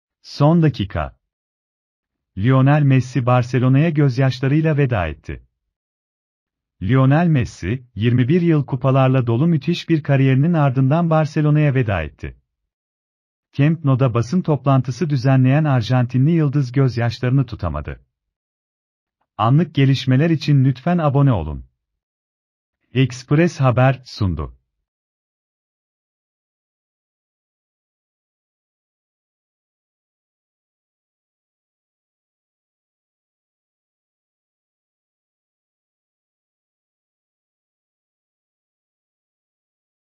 Son dakika... Lionel Messi Barcelona’ya gözyaşlarıyla veda etti
Lionel Messi, 21 yıl kupalarla dolu müthiş bir kariyerinin ardından Barcelona’ya veda etti. Camp Nou’da basın toplantısı düzenleyen Arjantinli yıldız gözyaşlarını tutamadı.